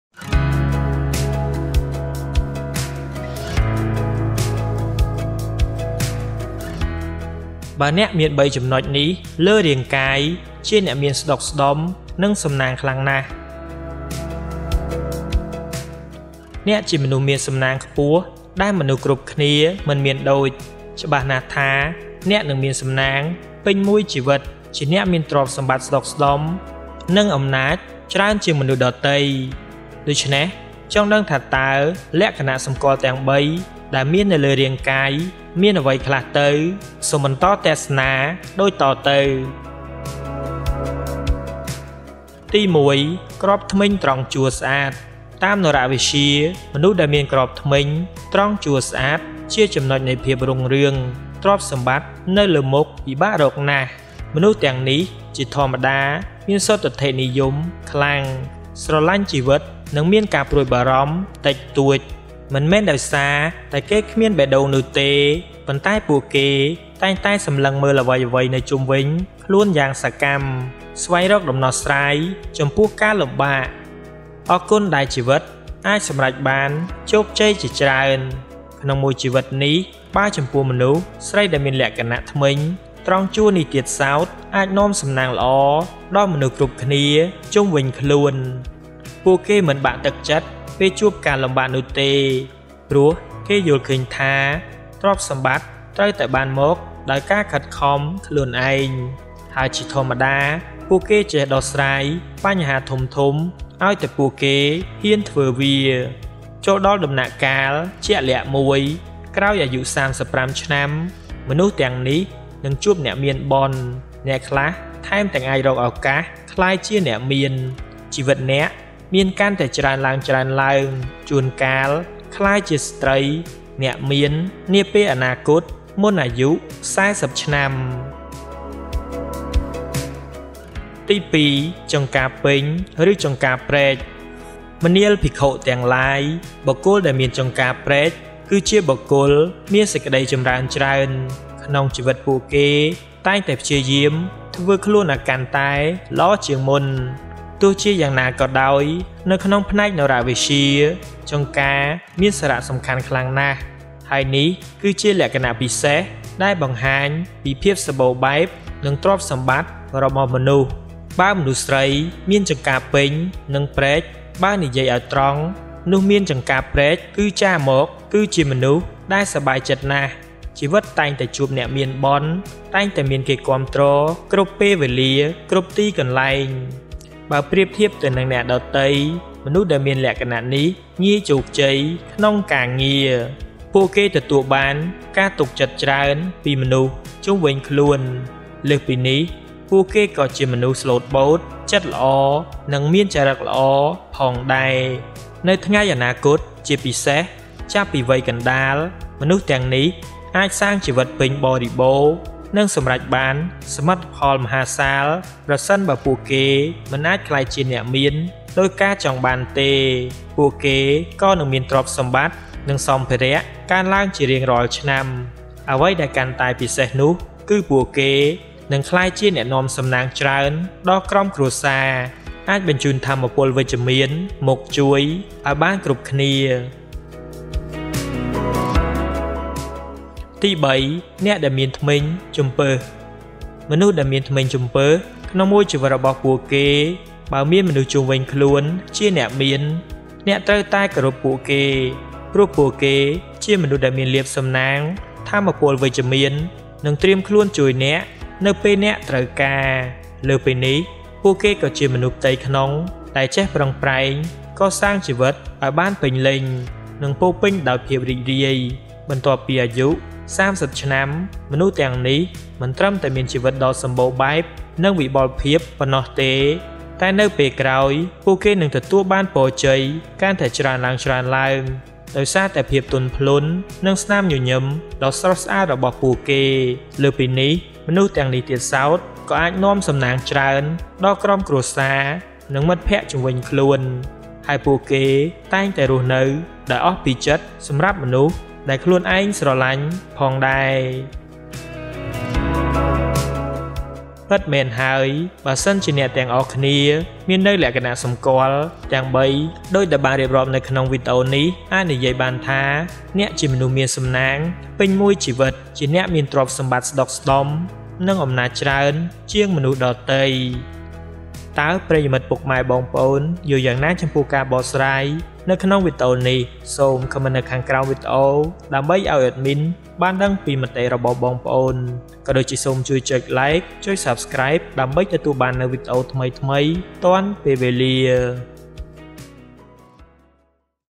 បើអ្នកមានចង្ការព្រែក និងធ្មេញជំពឹស ជាអ្នកមានស្ដុកស្ដម្ភ និងសំណាងខ្ពស់ | feng shui 2022 | Chouk Chet
👉 ចង់ឧបត្ថម្ភ Admin តាមរយៈ (ABA) : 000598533
Please make constructive comments for us to correct.